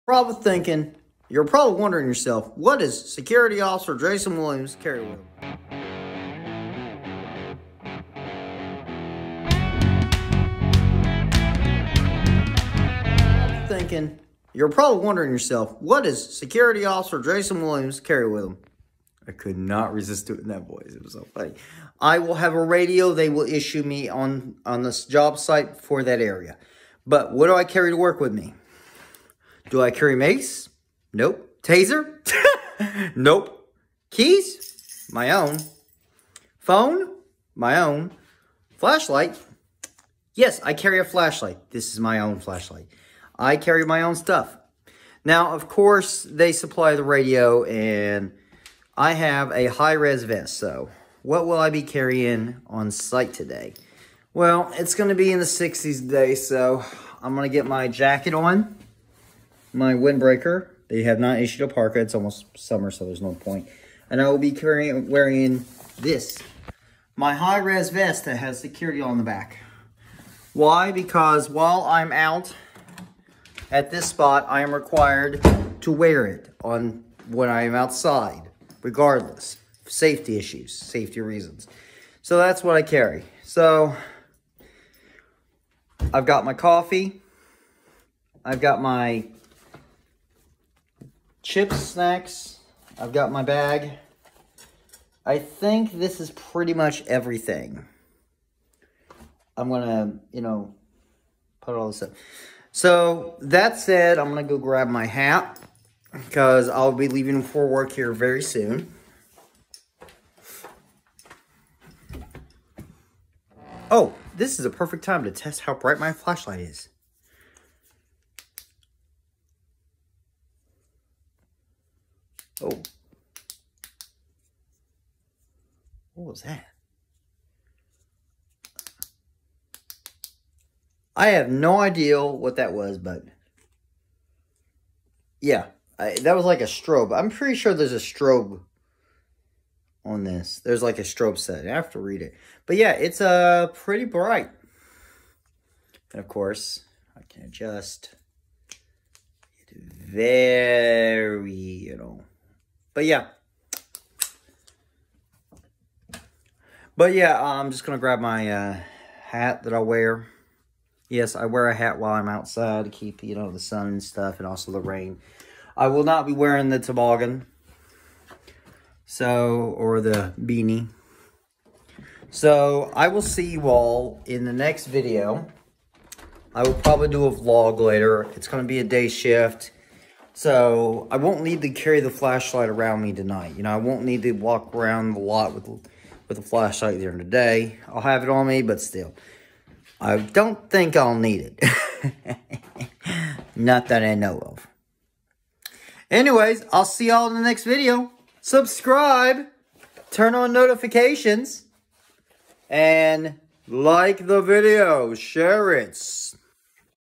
Security officer Williams Vlog 2 ￼
So what does a security officer carry to work with him. Well I don’t know about others but this is what I carry to work with me and what I have while on post. Enjoy the vlog.